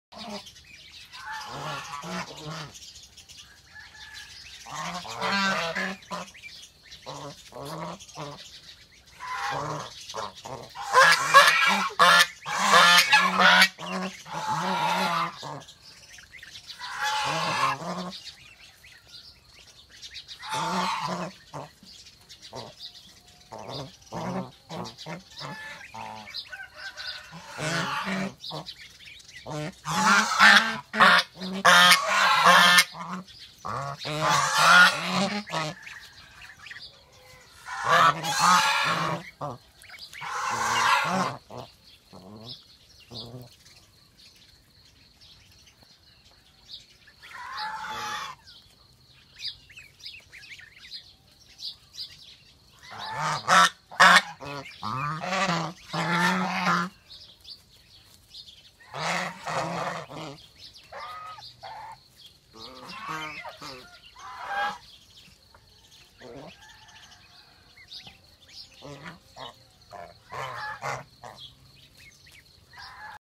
ក្ងានជាសត្វងាយចិញ្ចឹម
ក្ងានជាសត្វដែលងាយចិញ្ចឹមមិនរើសចំណី ចំណាយទន់តិច តែមិនសូវមានទីផ្សារ!